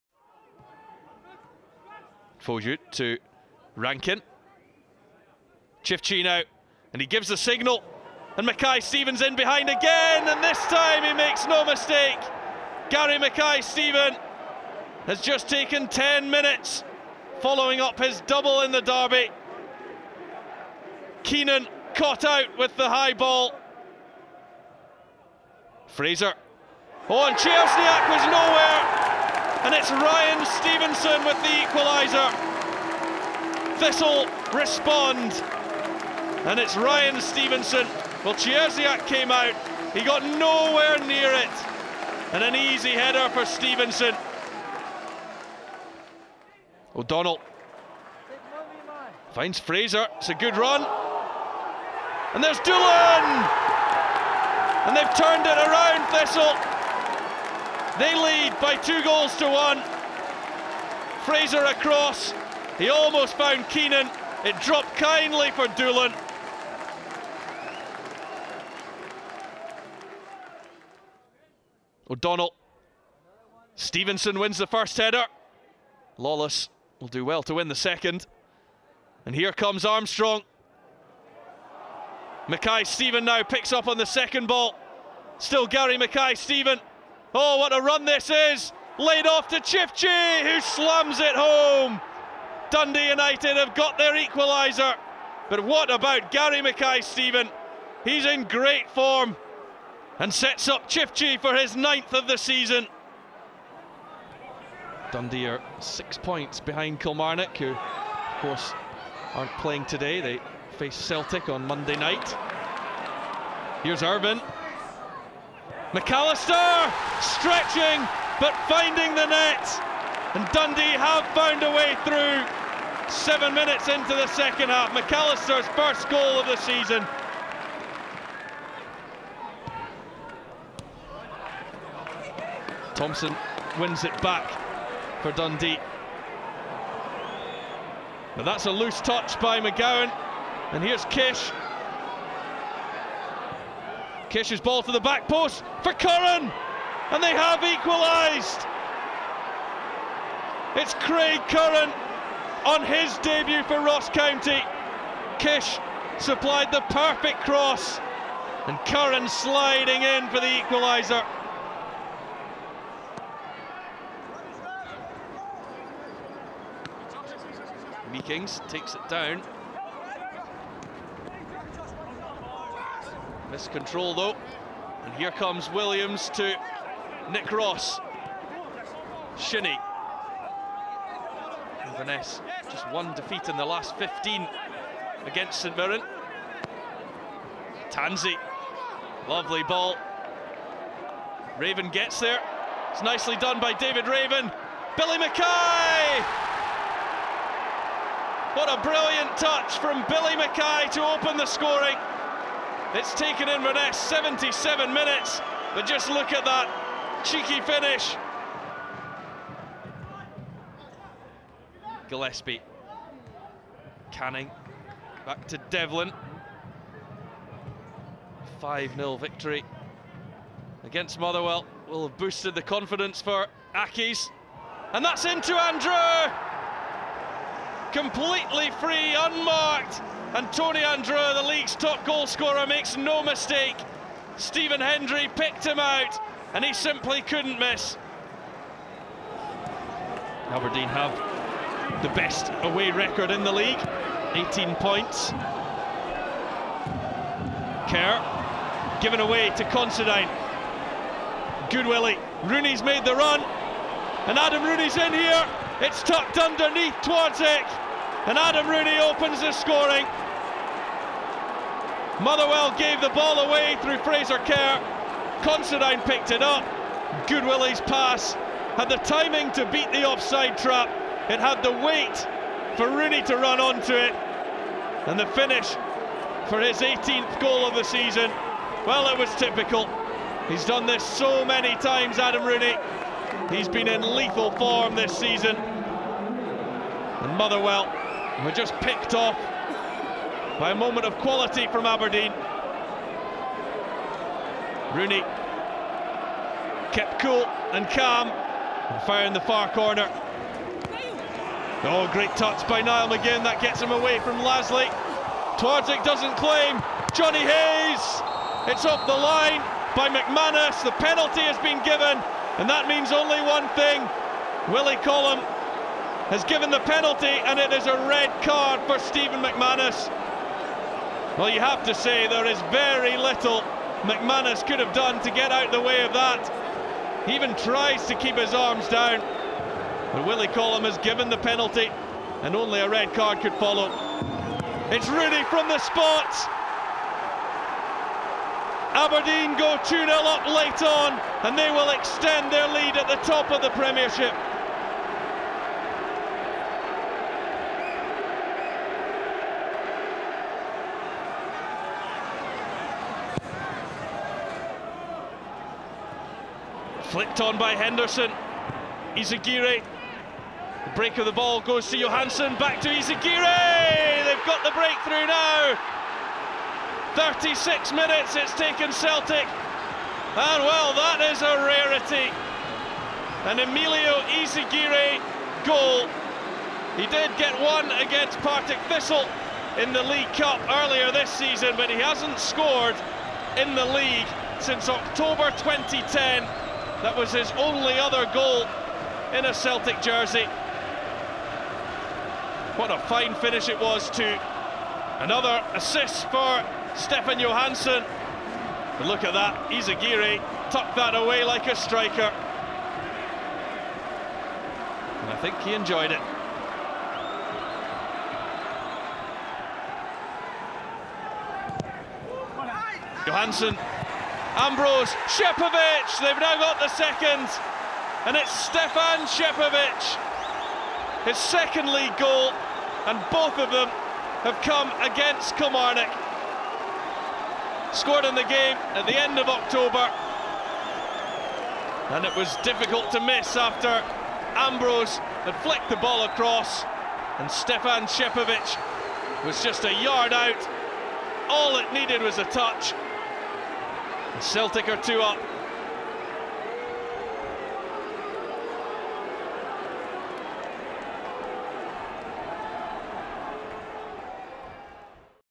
Watch every goal from the Scottish Premiership!
Watch every goal scored in every game played in the Scottish Premiership on Sunday 04/01/2015 and Monday 05/01/2015, featuring strikes from Gary Mackay-Steven, Ryan Stevenson, Kris Doolan, Nadir Ciftci, Tony Andreu, Jim McAlister, Craig Curran, Billy McKay, Adam Rooney, Emilio Izaguirre and Stefan Scepovic.